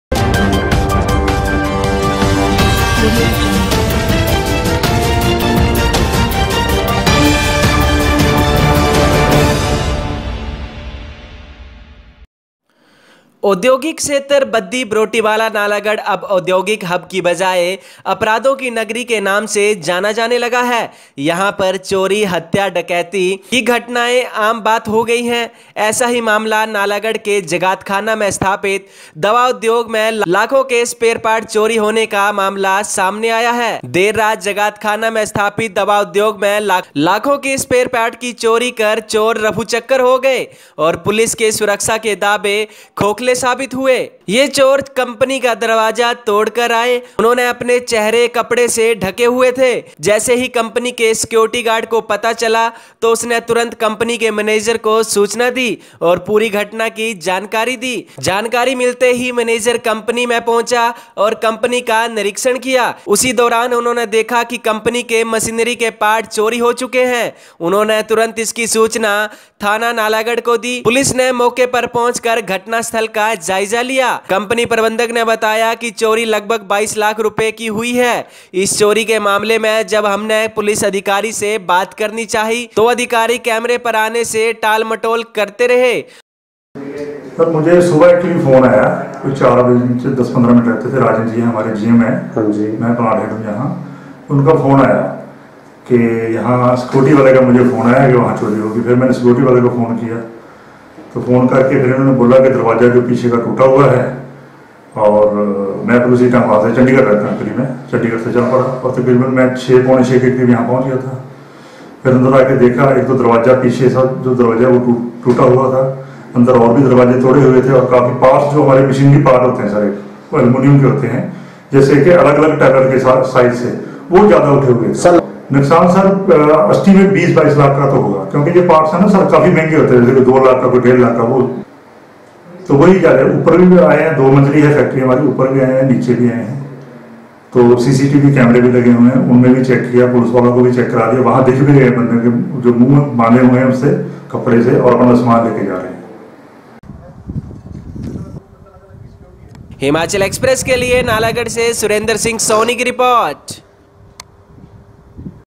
नालागढ़ के तहत जगातखाना में स्थापित दवा उद्योग में लाखों की चोरी
ओधोगिक क्षेत्र बद्दी बरोटीवाला नालागढ़ अब ओधोगिक हब की वजाये अपराधों की नगरी के नाम से जाना जाने लगा !यहाँ पर चोरी हत्या डकैती फरौती में चोरी की घटनाये होना आम बात हो गयी है